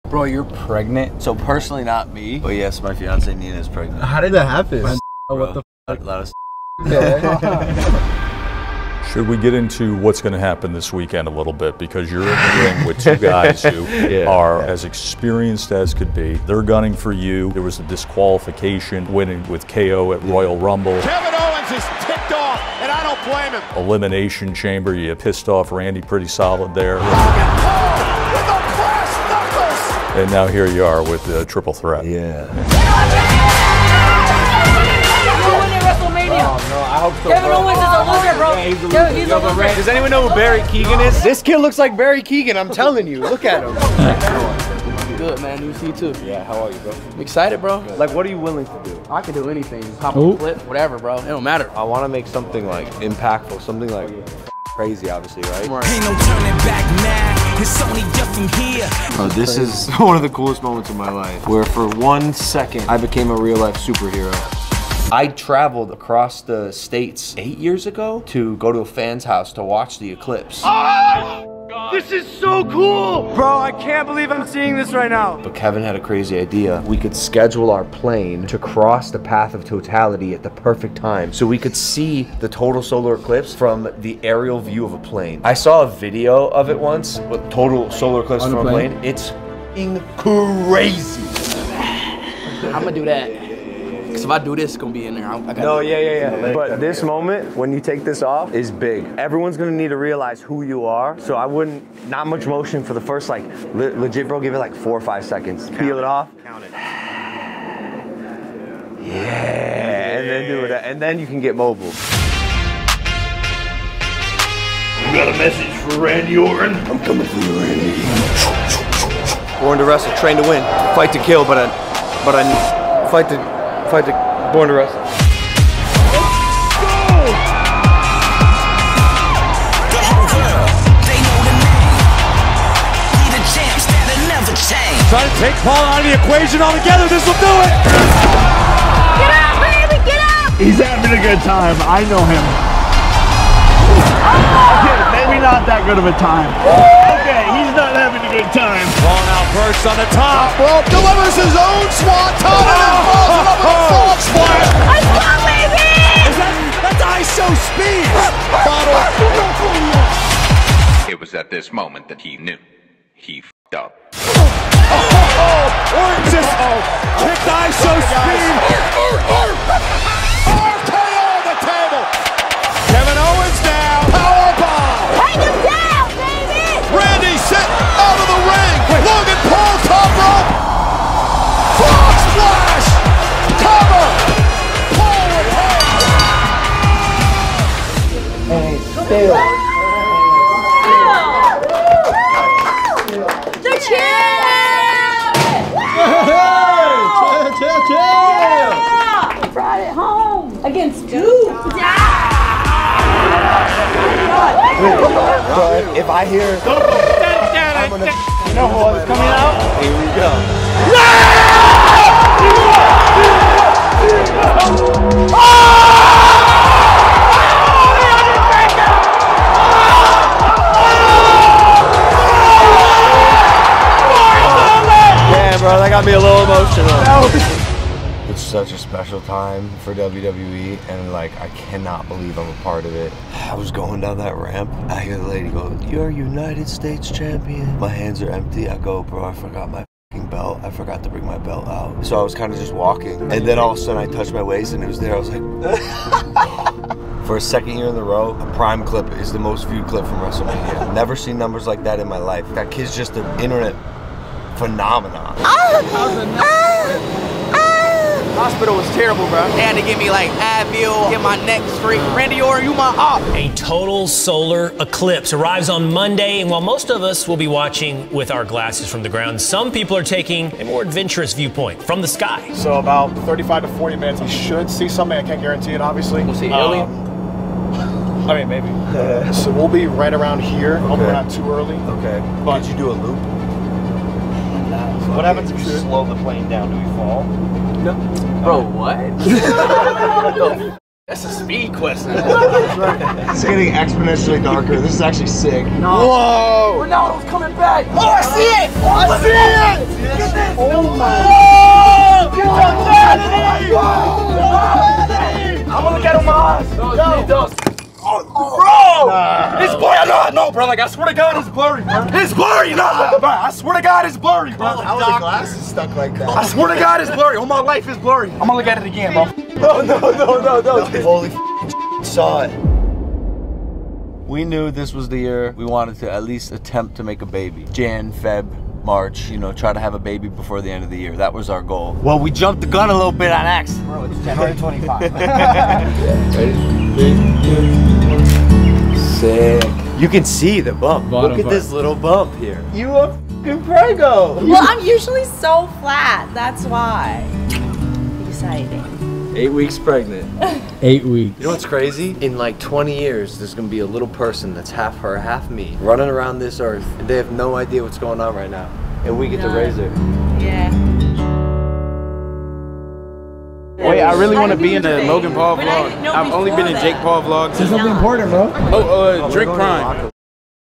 I’m Going To Be A Dad
Another Paul has entered the chat

I’m a 29 year old living in Puerto Rico. This is my life.